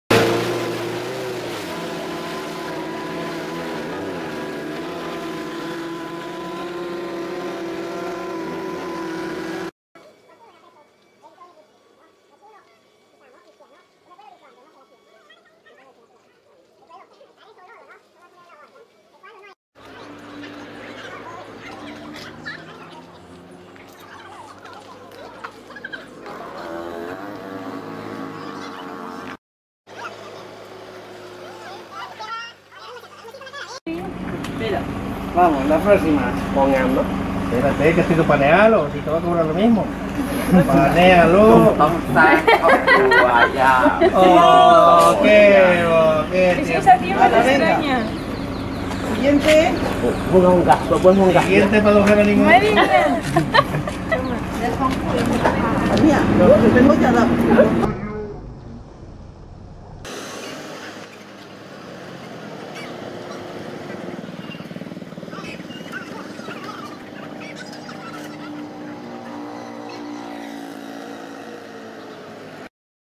A village on Nam Mae Kok, Thailand
Nam Mae Kok, The Kok River, originates in the Daen Lao Range, Shan State, Burma. Most of its length in Thailand is in Chiang Rai Province.